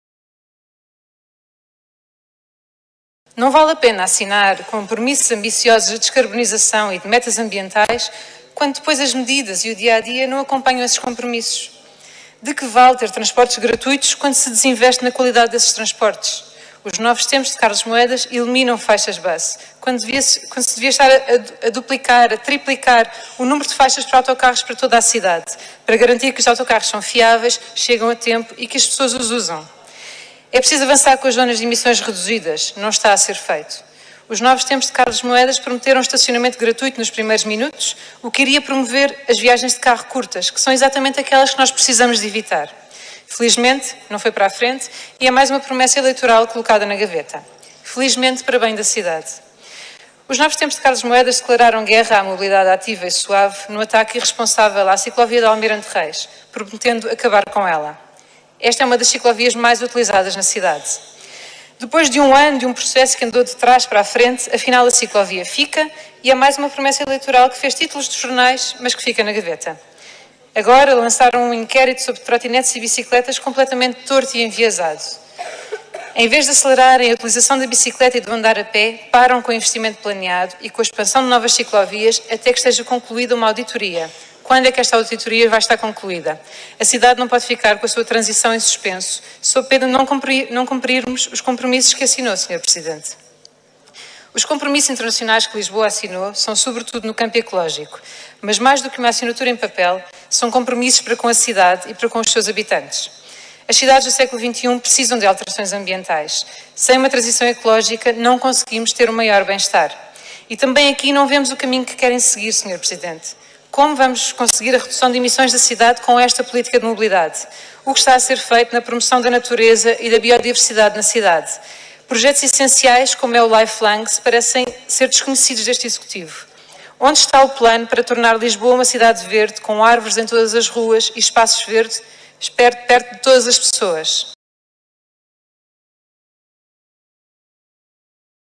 Recuo na mobilidade e na ecologia em Lisboa
"São antiquadas e irresponsáveis as políticas de mobilidade e ambientais dos Novos Tempos de Carlos Moedas em Lisboa. Como vamos cumprir os compromissos internacionais e servir as pessoas?"  — Isabel Mendes Lopes do LIVRE no debate do Estado da Cidade.

Assembleia Municipal de Lisboa, 11 de outubro de 2022